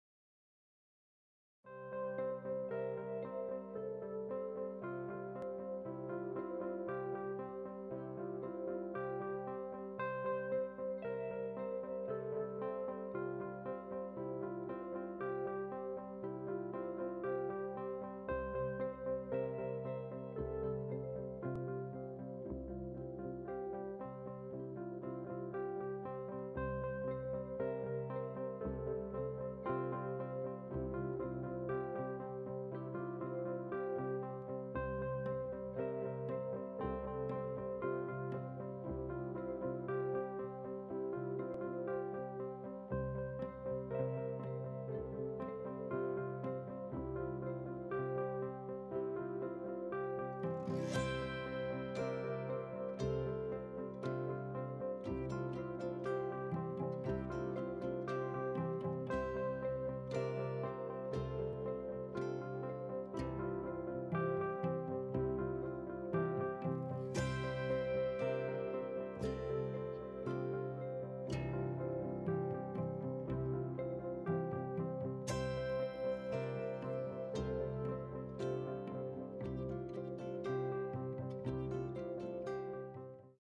Painting for Pleasure - Landscapes (Part 2) Inspired by WowArt
These are a set of landscape paintings which have been inspired by the beautiful painting videos of Wow Art Channel. Below are the links to the full tutorials of each of the paintings: